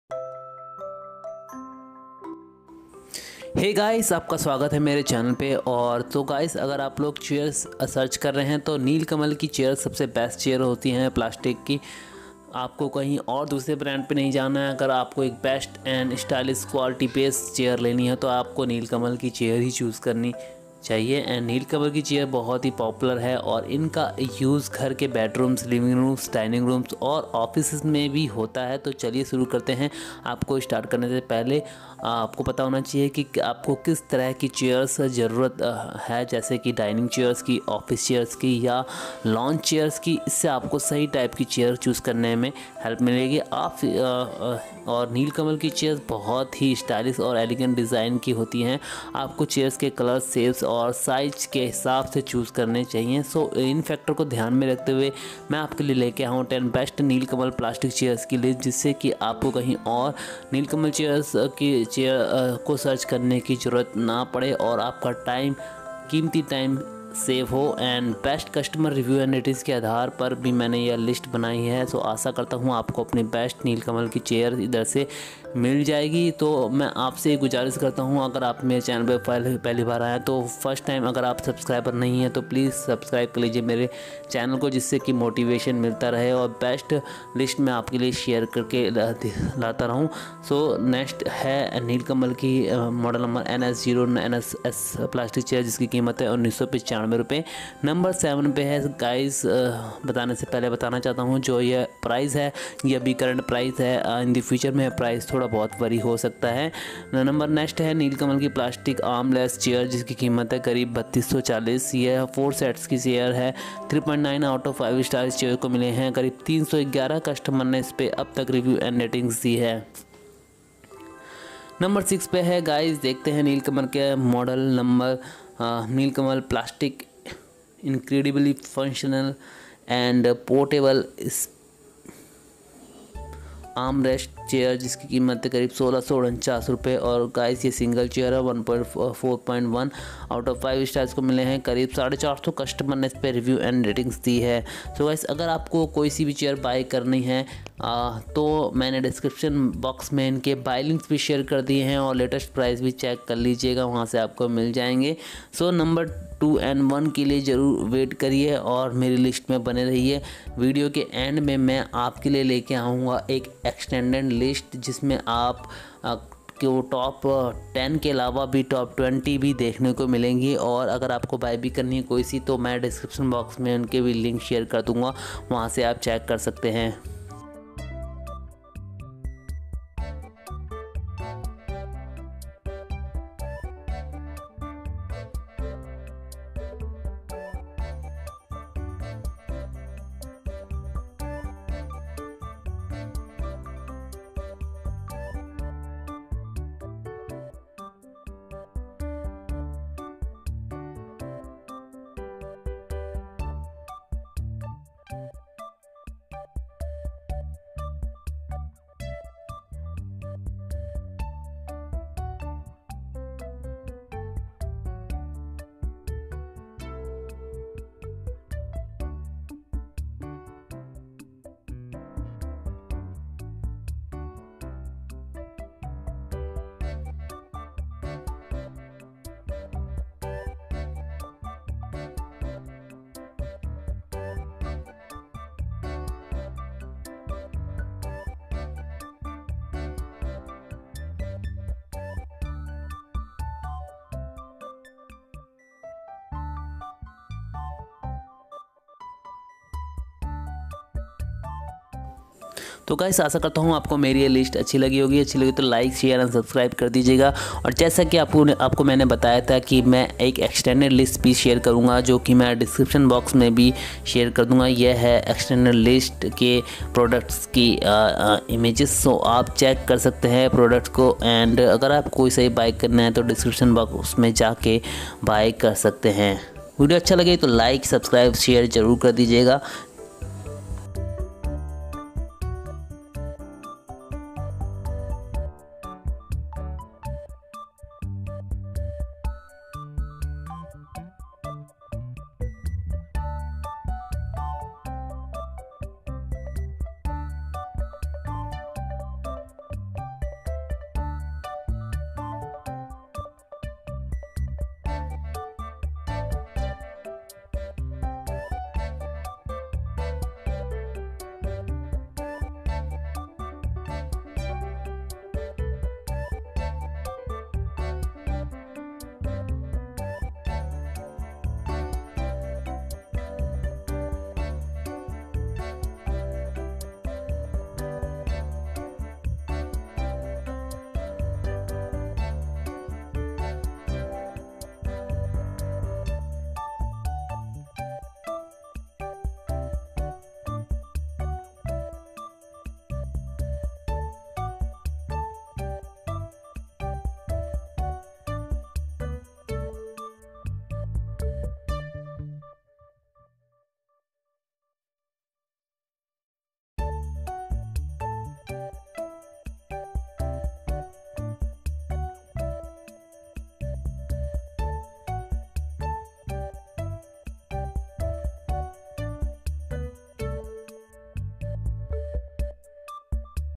The 10 Best Nilkamal Plastics Chairs in India till 2024 | Durable Plastics Chairs Under 2000
Checkout The 10 Best Nilkamal Plastics Chairs in India till 2024 | Durable Plastics Chairs Under 2000
(10) : Nilkamal Mystique Chair Charcoal Grey ▶️
(9): Nilkamal Plastic Incredibly Functional Spinecare ▶️
(8): Nilkamal Back Plastic Chair Brown ▶️
(7): Nilkamal Eeezy Chair Weather Standard ▶️
(6): Nilkamal 2180 Plastic Chair Brown New ▶️
(5): Nilkamal CHR2226 Season Biscuit Standard ▶️
(4): NILKAMAL Crystal Chair Garden White ▶️
(3): Nilkamal Weekender Chair Weather Brown ▶️
(2): Nilkamal CHR2051 Plastic Chair Pear ▶️
(1): Nilkamal Novella Stainless Steel Armchair ▶️

Extended List of Plastic Chairs

(11): Nilkamal Plastic Armless Chair 39x47x89cm ▶️
(12): Nilkamal Mistique Premium Chair pack ▶️
(13): Nilkamal 2180 Plastic Chair Brown ▶️
(14): Nilkamal Plastic Premium Chair Brown ▶️
(15): Nilkamal Plastic Premium Mystique Chair ▶️

Thanks